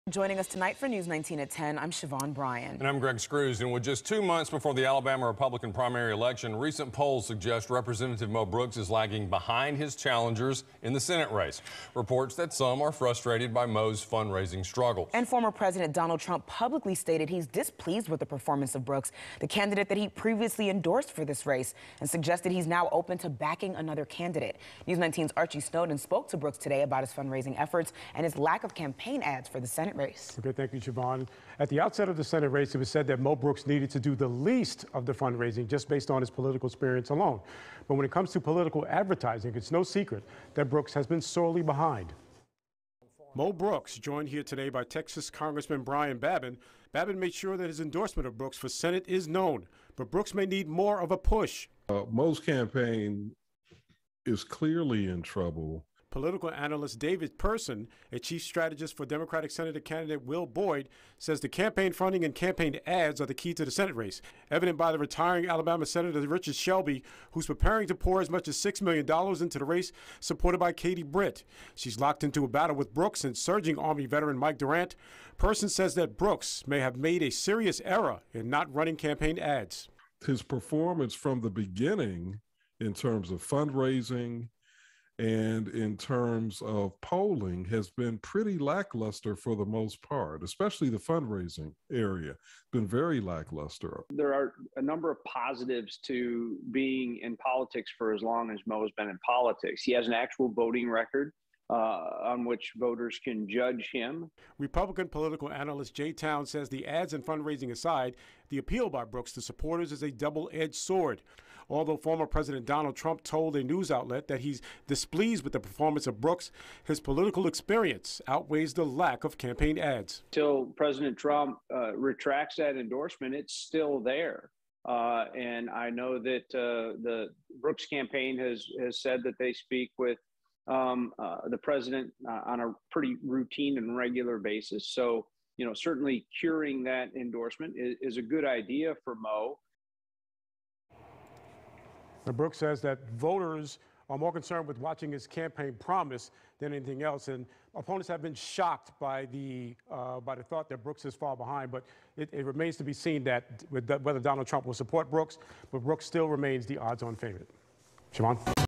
Mo Brooks Senate Race Ads Begin
With just two months before the Alabama Republican Primary election, polls suggest Representative Mo Brooks is lagging behind his challengers in the senate race. Reports say that some are frustrated by Mo's fundraising struggles.